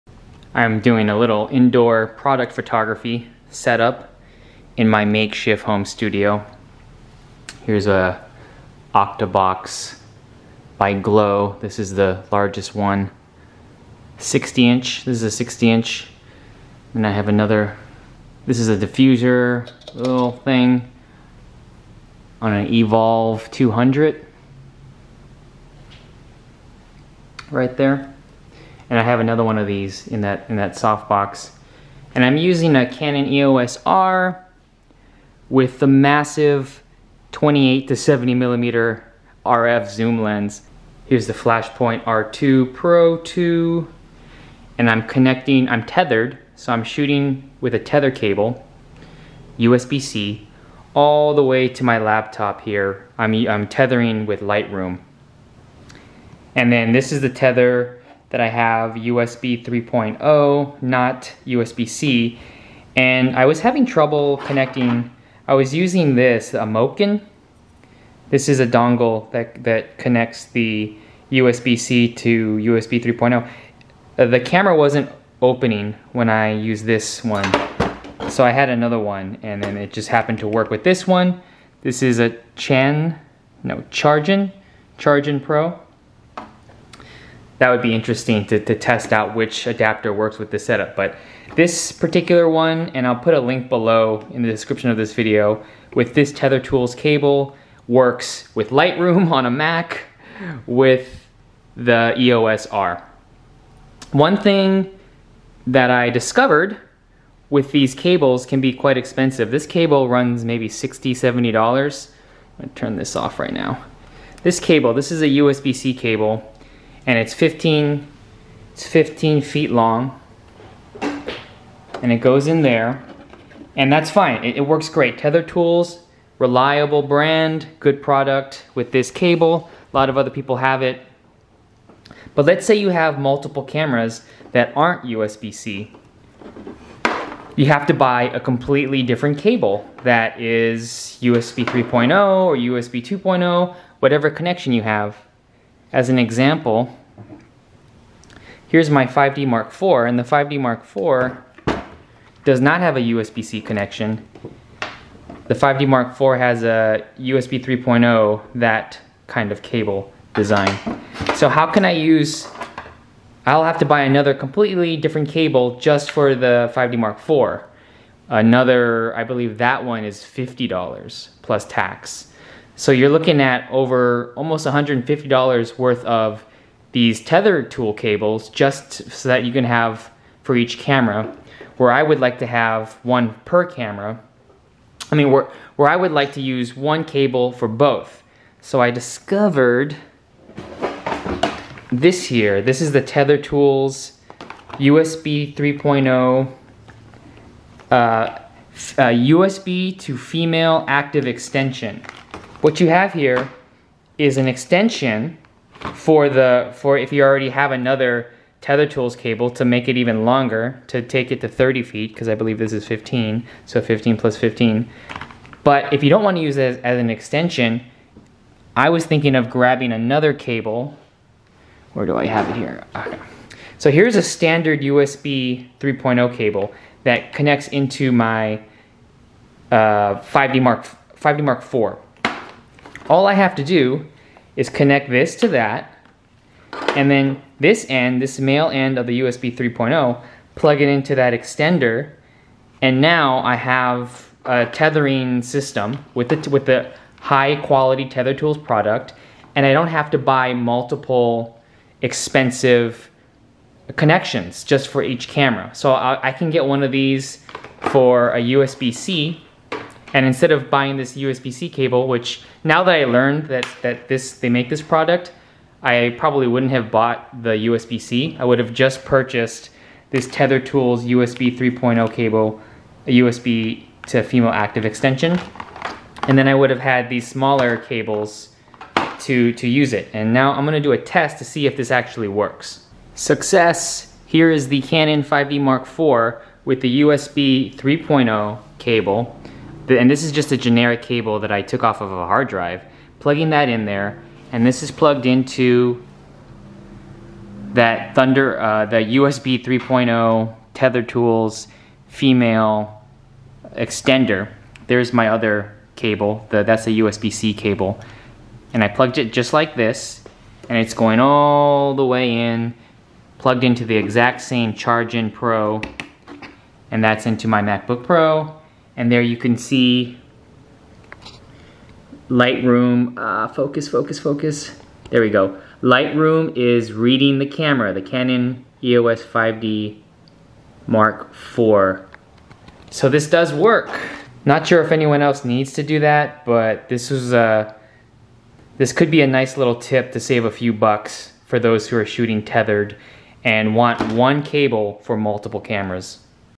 Tether Tools TetherPro USB 3.0 cable | Lightroom | Canon EOS R
Canon EOS R and Canon 5D Mark IV into Adobe Lightroom Classic with Tether Tools TetherPro cables.
Tether Tools TetherPro USB 3.0 cable
CharJenPro USB C Hub

My 2021 Photography Kit:
Bodies
Canon EOS R
Canon 5D Mark IV

Adapters/Filters:
Canon EF-EOS R Adapter
Canon EF-EOS R Drop-in Filter with VariND